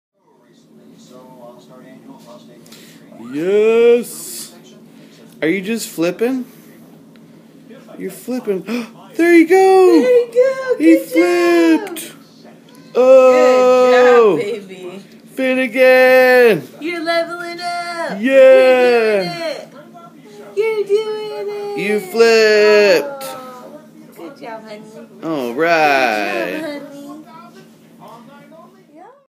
Flippin Finn
Finnigan Flips over for the first time.